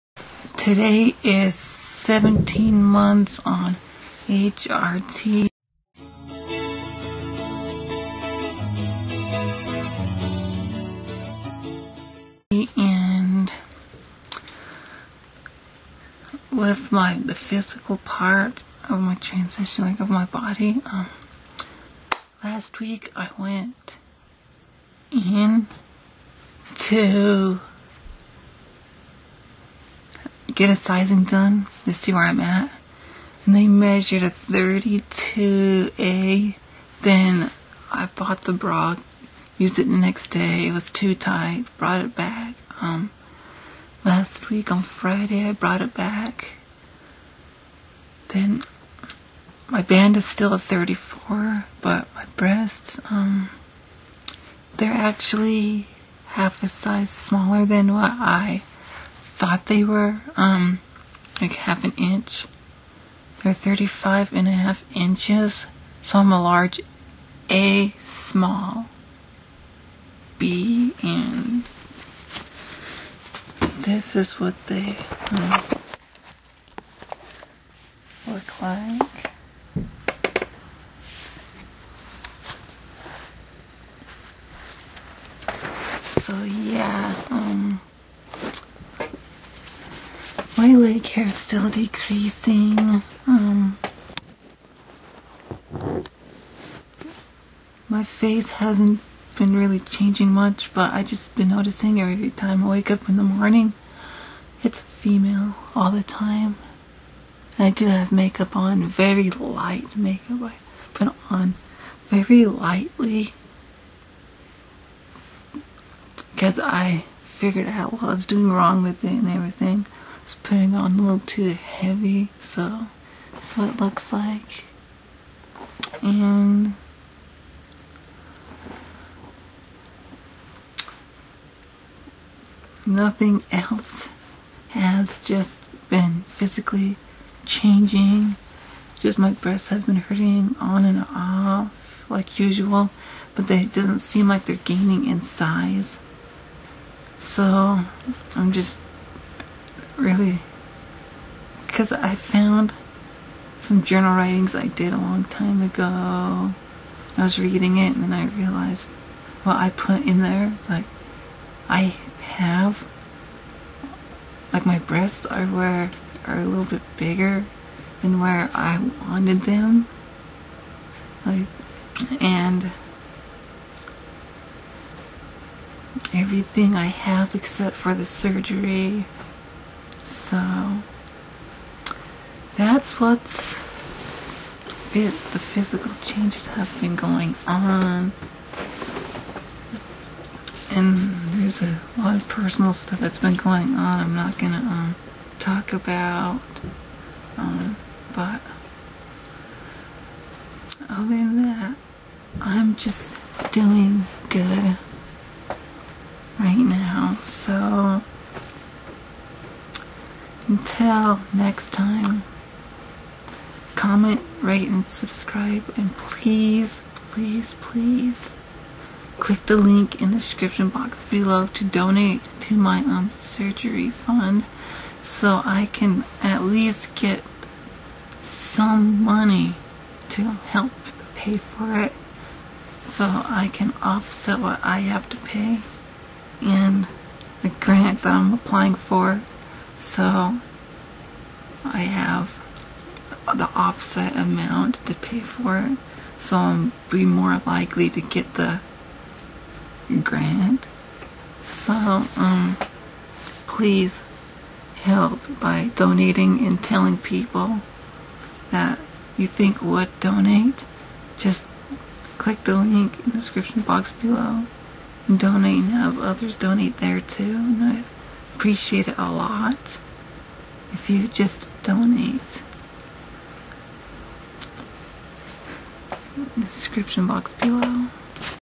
week 70 17 months HRT 515 days 4-18-12.wmv
week 70 17 months update on (HRT) hormone replacement therapy.